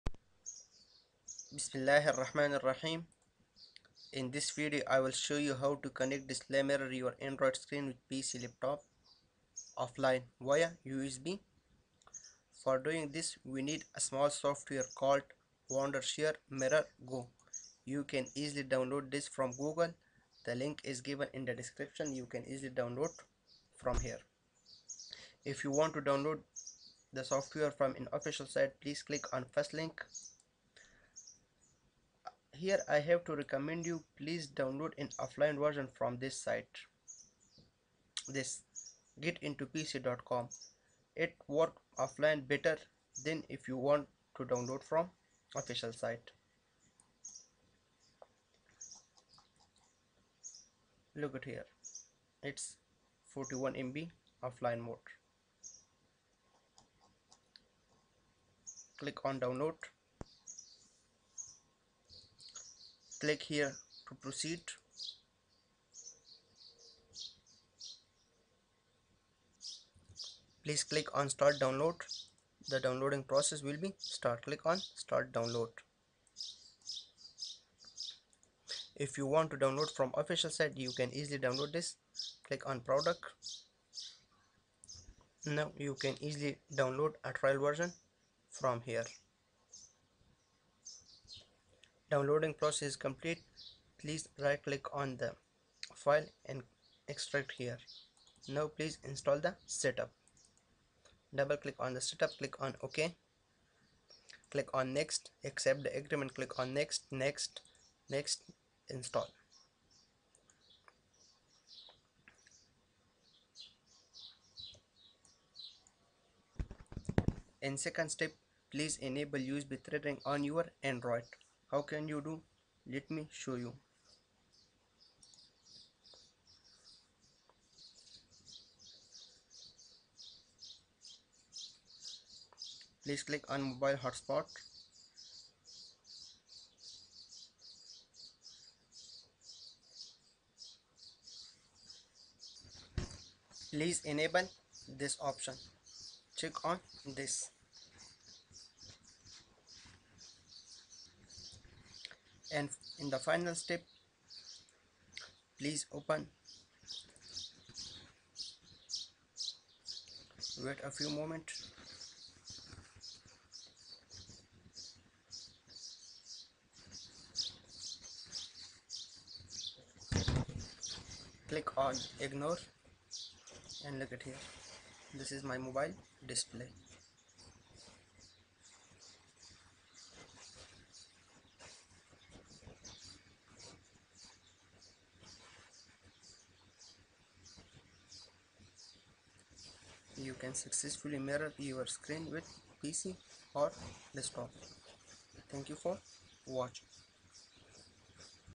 How to Connect/ Display/ Mirror Android Screen With PC Offline Via USB
How to Connect Any Android Product Display /Screen with PC/Laptop Via USB port or Wireless

haroonamin

►Skype :-
Haroon_amin22